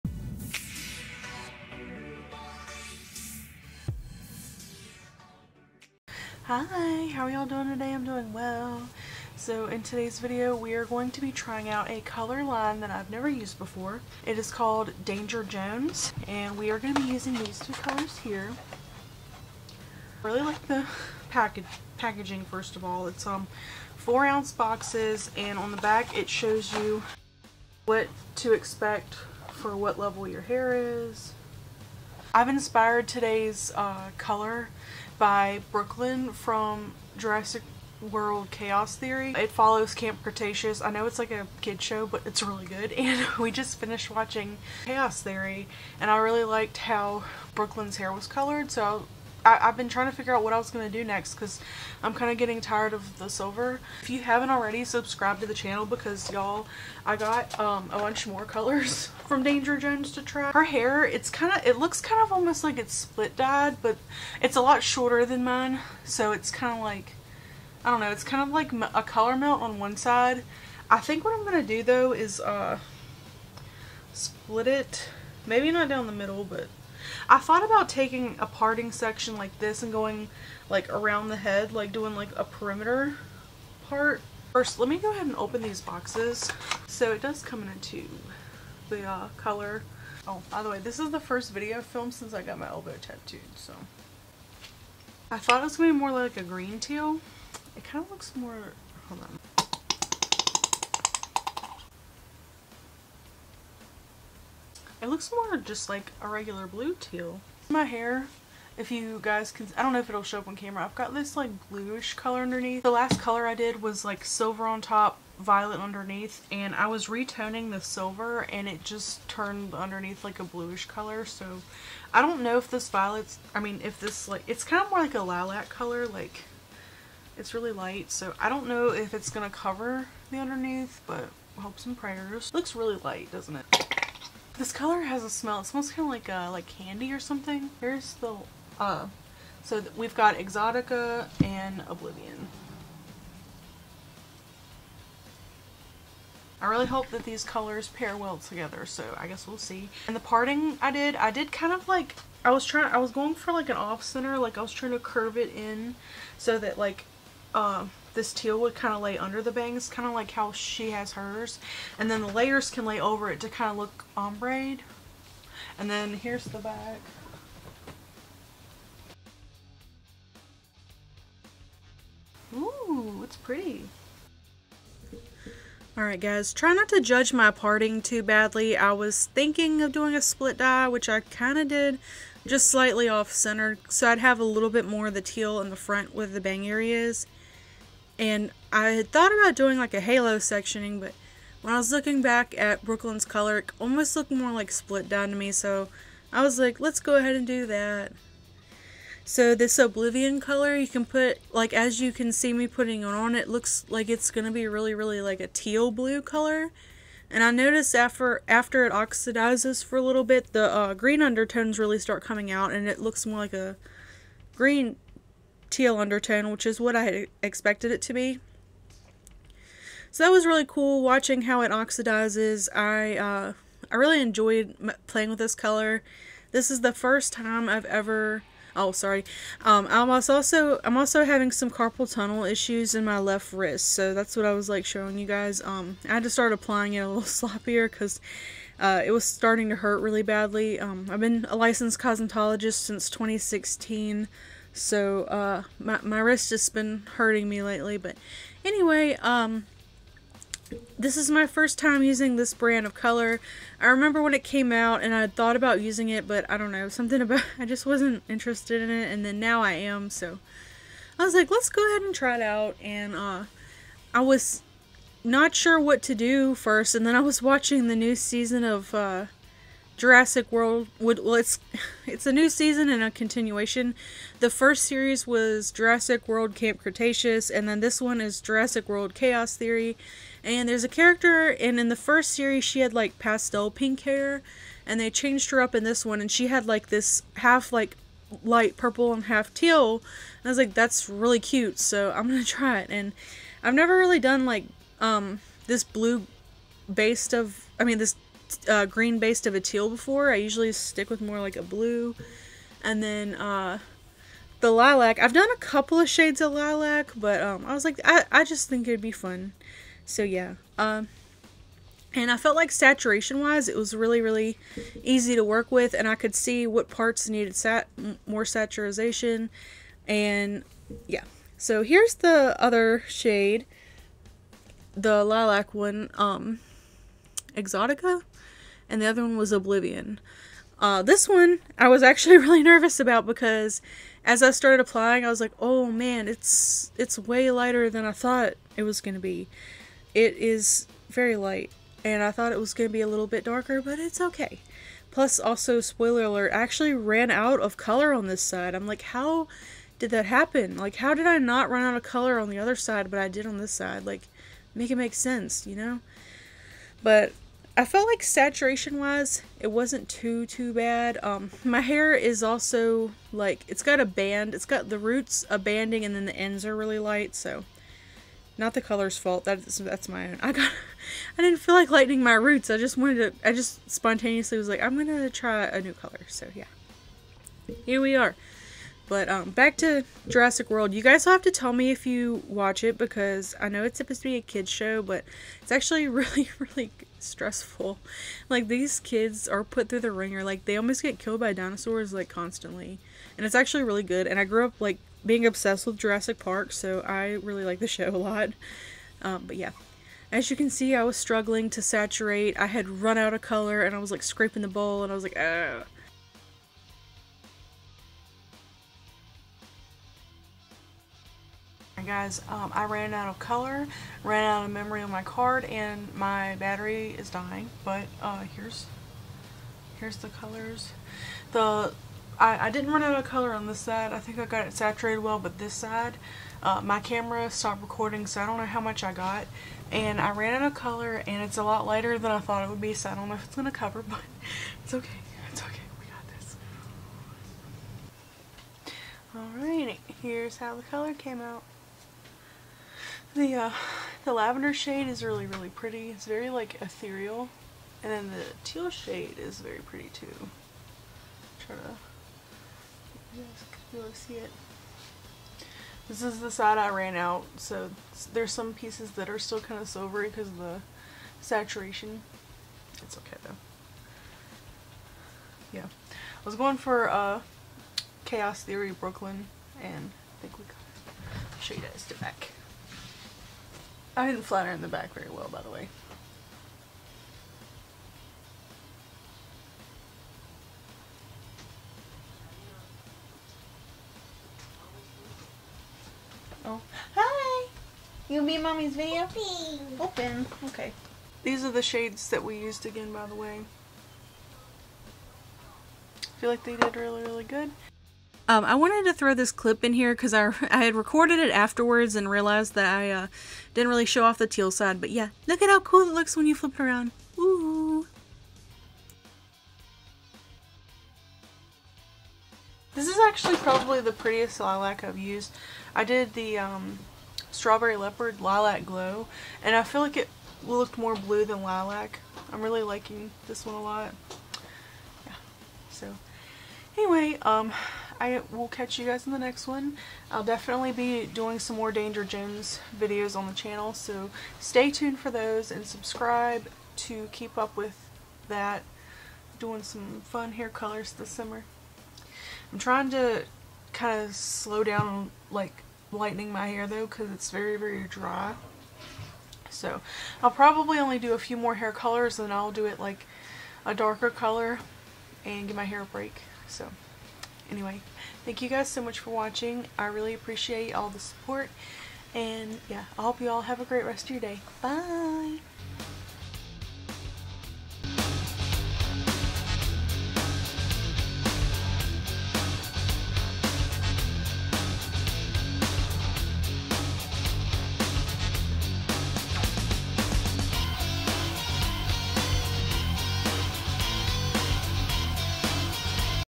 Danger Jones Oblivion & Exotica Split Dye (Jurassic World Brooklynn Inspired Hair Color)
Danger Jones came out a while back and today I am finally trying it out! I inspired the color from a character named Brooklynn from the Jurassic World animated series! How do you guys think it turned out?